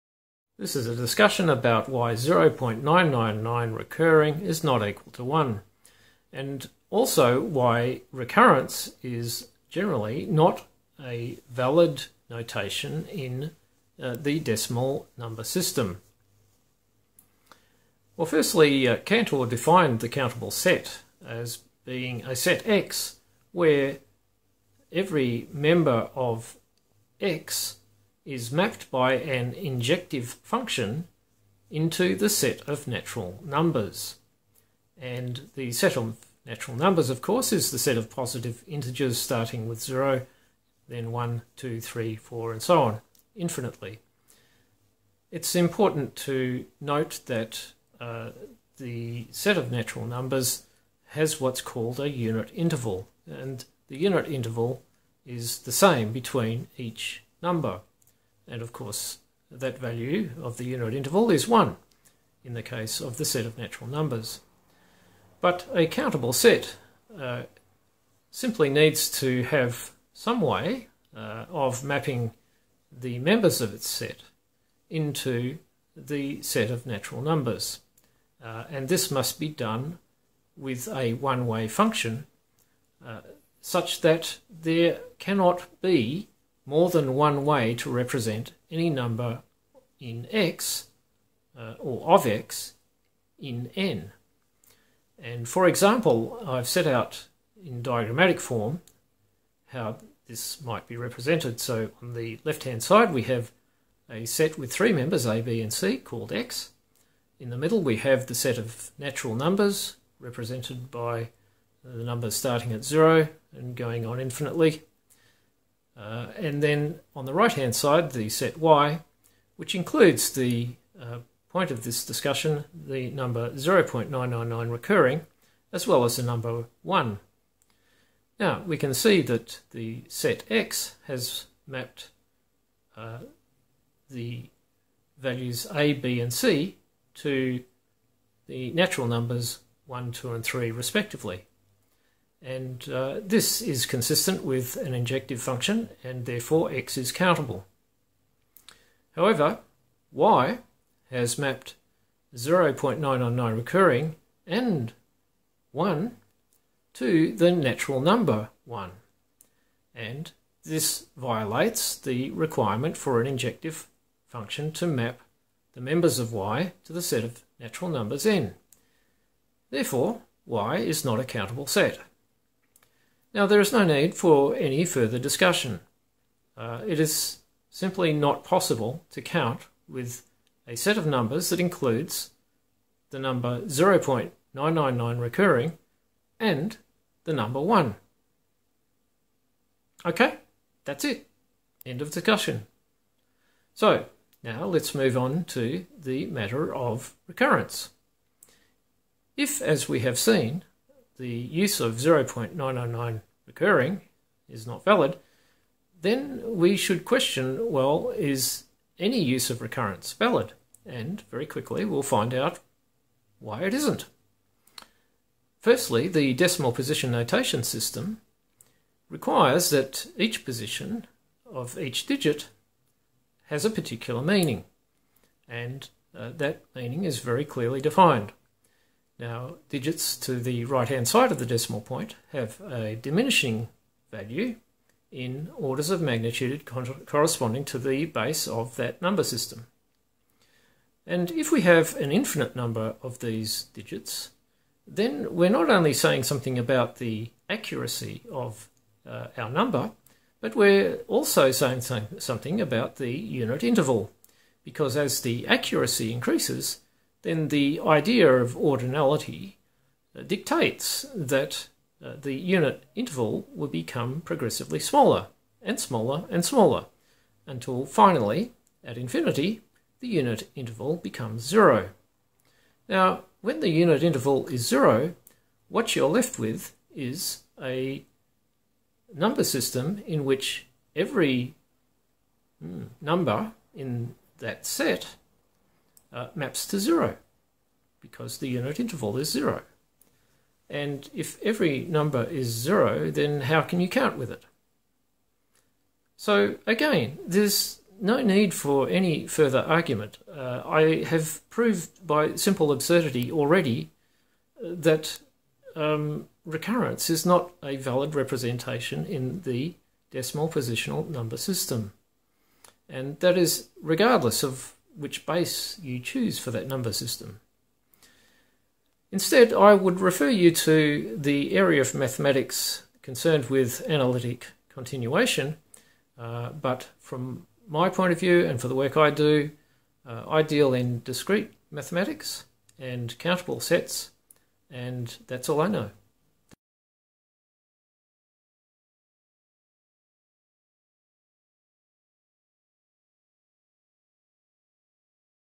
0.999... is NOT 1 - Here's why - Cantor's proof
Why 0.999… is NOT equal to 1 and RECURRENCE is NOT valid in the decimal notation system

Georg Cantor showed that a Countable Set X must have
Injective (one to one) function F:X to N
where N is the set of Natural numbers = {0,1,2,3,…}
One way function f maps every member of X to a single member of N
There can NOT be “more than one way” to map  the members of X to the same member in N
Therefor 0.999... = 1 makes the number set uncountable.

Recurrence
Decimal positional number system
Each digit’s position has meaning
Finite number of digits
Rightmost digit implies precision and therefore UNIT INTERVAL
Discrete mathematics does not represent every real number
Recurrence implies infinite number of digits, infinite precision, unit interval of ZERO
Therefore every number is ZERO
NOT a countable set
NOT even Discrete math